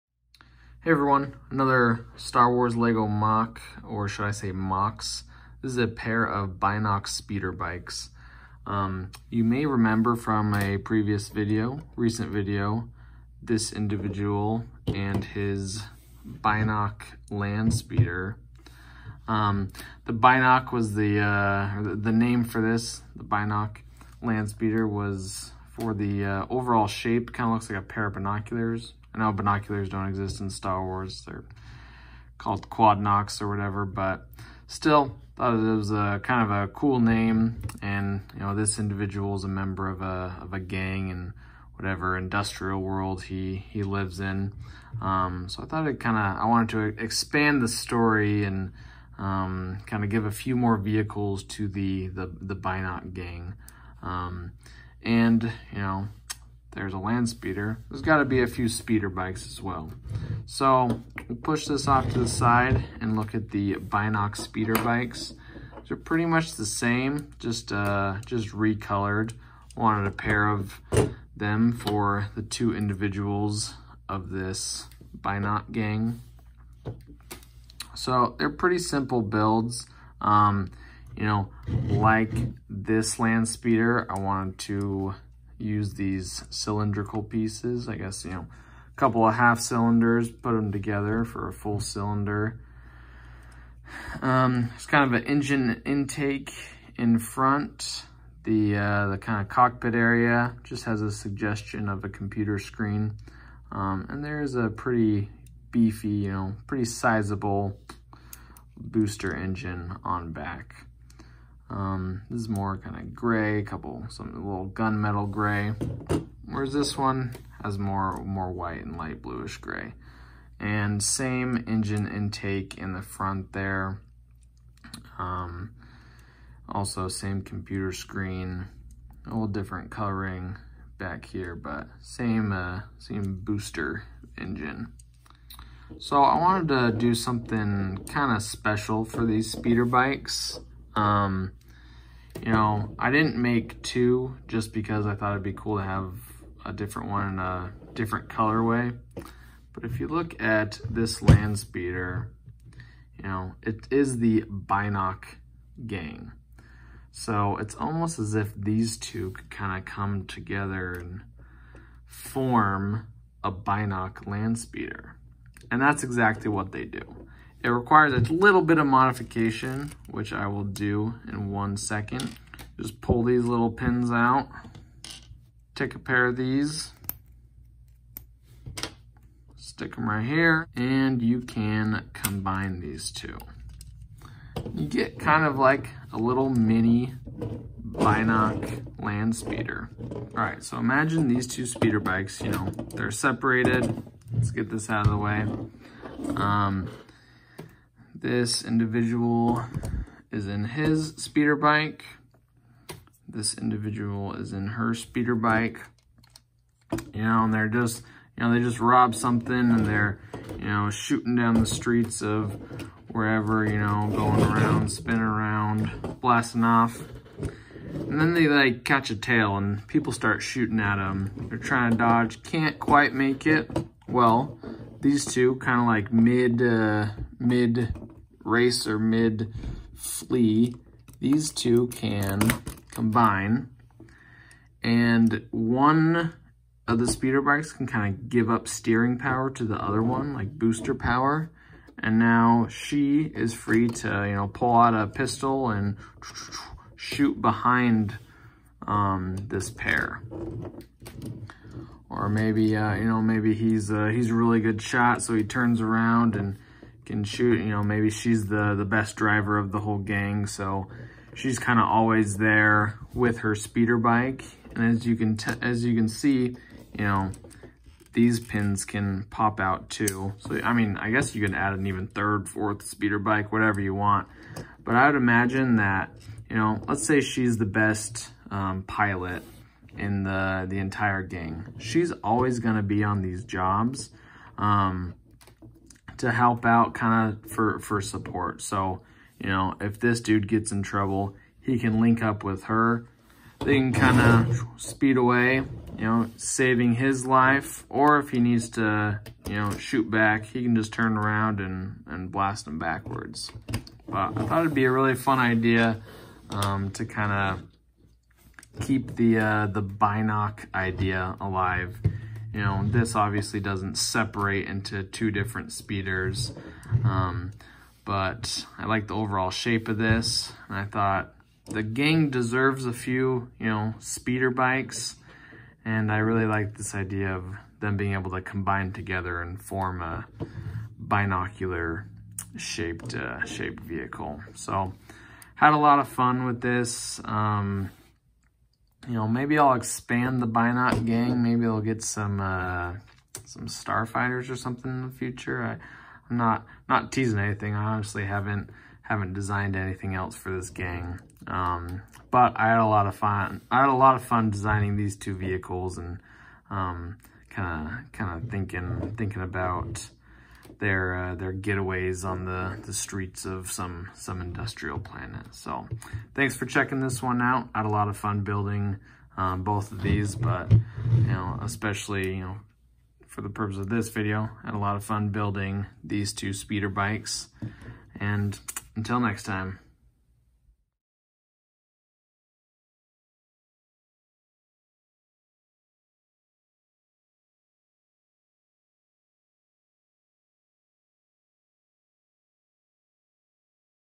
Star Wars Lego MOC: Binoc Speederbikes
Couple additional vehicles for the Binoc Gang